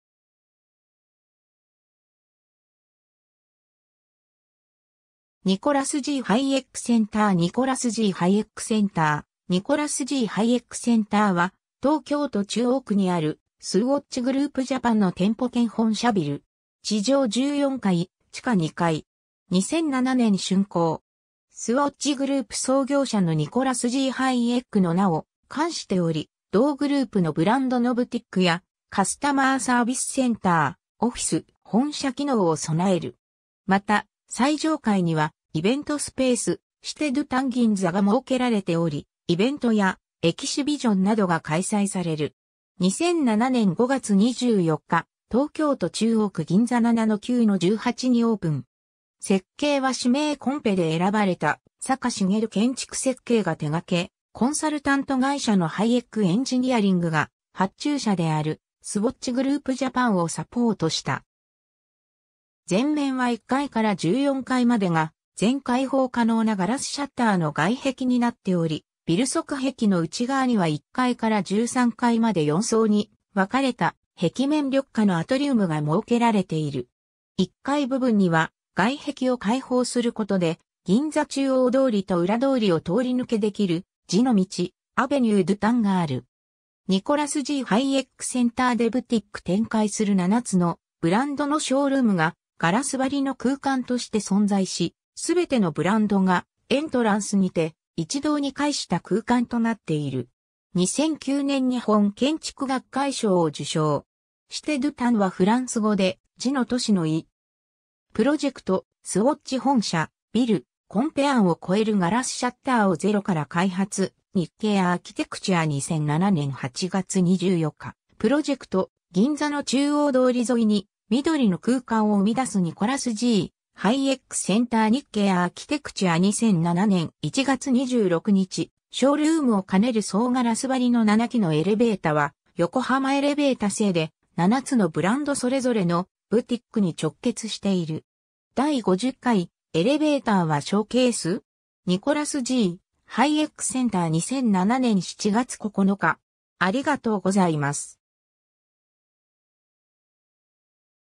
ニコラス・G・ハイエックセンター
スウォッチ・グループ
ニコラス・G・ハイエックセンター ニコラス・G・ハイエックセンター ニコラス・G・ハイエックセンター (Nicolas G. Hayek Center) は、東京都中央区にある、スウォッチ・グループジャパンの店舗兼本社ビル。
地上14階、地下2階。
2007年竣工。
 スウォッチグループ創業者のニコラス・G・ハイエックの名を冠しており、同グループのブランドのブティックや、カスタマーサービスセンター、オフィス・本社機能を備える。
また、最上階にはイベントスペース「シテ・ドゥ・タン ギンザ」が設けられており、イベントやエキシビジョンなどが開催される。
 2007年5月24日、東京都中央区銀座7-9-18にオープン。
設計は指名コンペで選ばれた坂茂建築設計が手がけ、コンサルタント会社のハイエック･エンジニアリングが発注者であるスウォッチグループジャパンをサポートした。
前面は1階から14階までが全開放可能なガラスシャッターの外壁になっており、ビル側壁の内側には1階から13階まで4層に分かれた壁面緑化のアトリウムが設けられている。
1階部分には、外壁を開放することで銀座中央通りと裏通りを通り抜け出来る“時の道”「アベニュー・ドゥ・タン」がある。
ニコラス・G・ハイエックセンターでブティック展開する7つのブランドのショールームがガラス張りの空間として存在し、全てのブランドがエントランスにて一堂に会した空間となっている。
2009年日本建築学会賞(作品賞)を受賞。
 ^ 「シテ・ドゥ・タン」はフランス語で「時の都市」の意。
 ^ 【プロジェクト】スウォッチ本社ビル、コンペ案を超えるガラスシャッターをゼロから開発　日経アーキテクチュア　2007/08/24 ^ 【プロジェクト】銀座の中央通り沿いに緑の空間を生み出す　ニコラス G. ハイエック センター　日経アーキテクチュア　2007/01/26 ^ ショールームを兼ねる総ガラス張りの7基のエレベータは横浜エレベータ製で、７つのブランドそれぞれのブティックに直結している。
 ^ 第50回「エレベーターはショーケース　?ニコラス・G・ハイエック センター」2007年7月9日